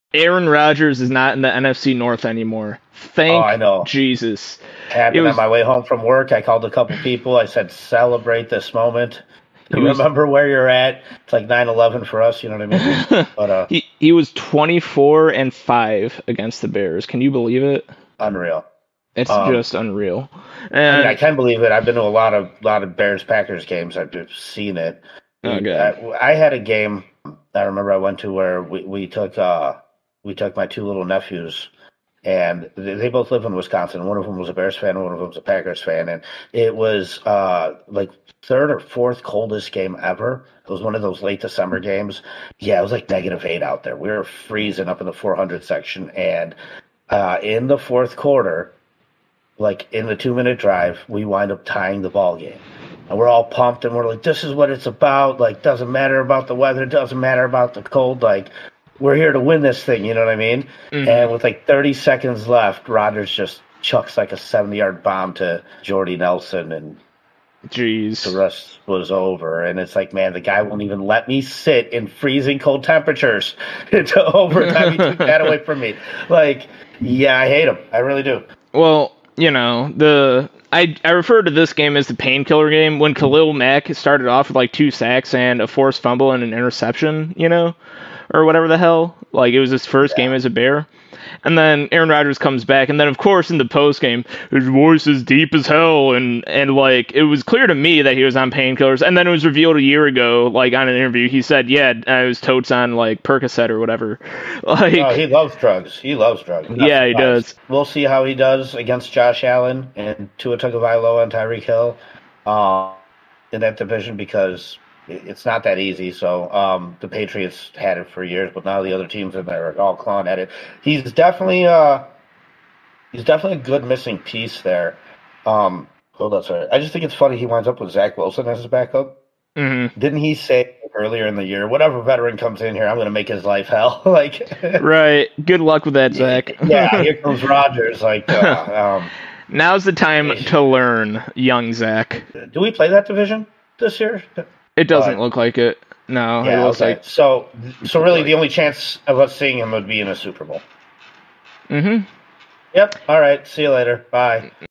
Aaron Rodgers FINALLY GONE - Reflecting on the Rodgers Trade to the Jets
Aaron Rodgers FINALLY GONE - Reflecting on the Rodgers Trade to the Jets. This was the domino that needed to fall for the NFC North to become open for the taking. Its a new era for Bears, Lions, Vikings, and Packers fans as well. This division is going to be for the taking! It just seems fitting that by the end of his career, even his own fan base didnt want him anymore. Bye Rodgers, dont let the door hit ya on the way out!